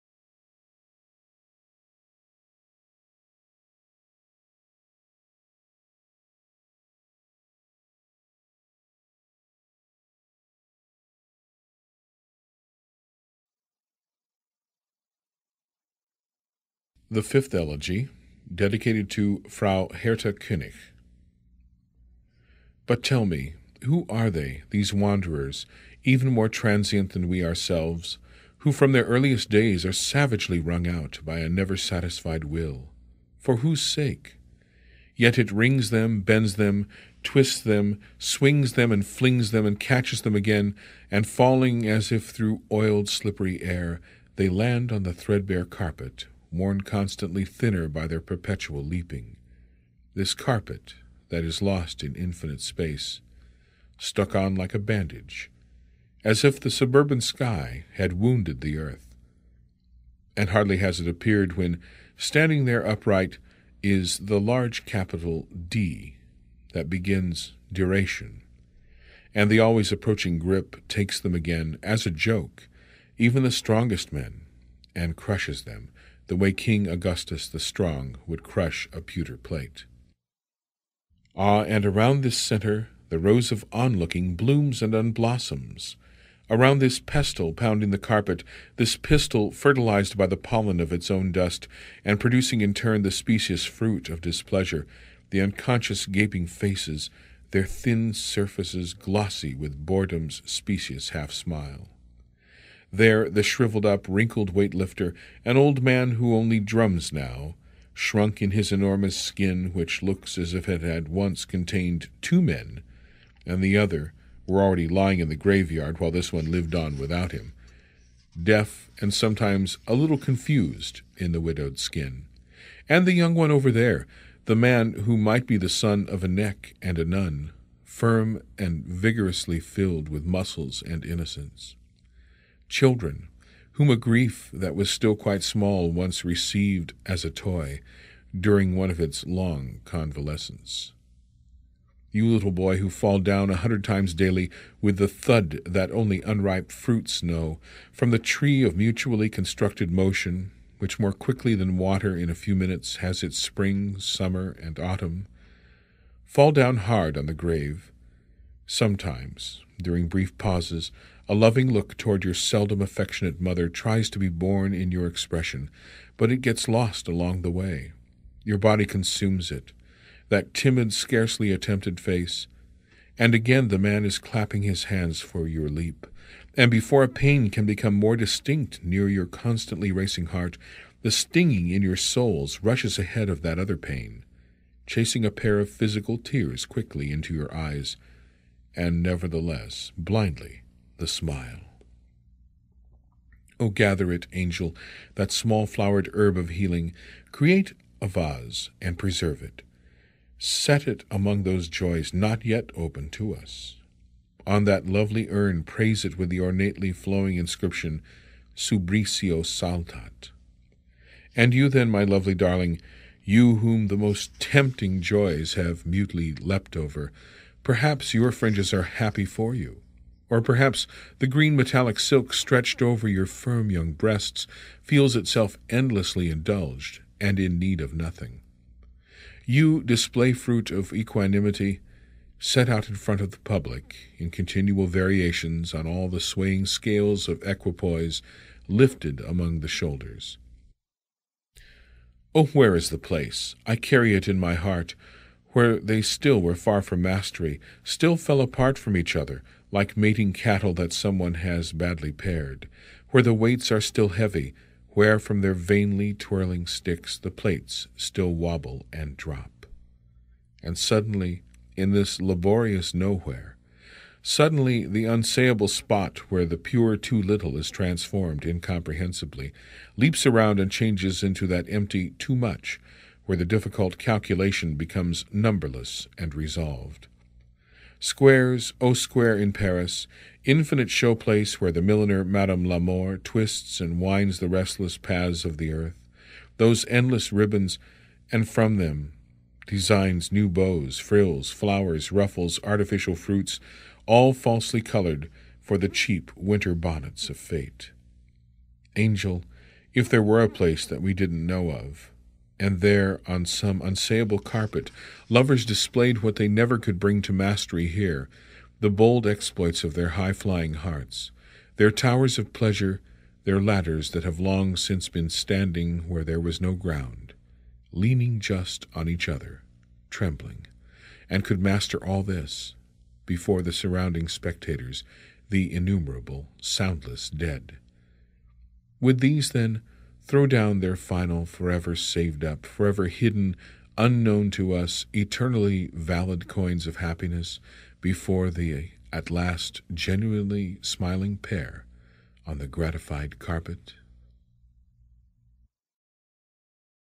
Duino Elegy No 5 by Rainer Maria Rilke -- Reading by Doug Ordunio
Translation is by Stephen Mitchell. I own no copyright of his words.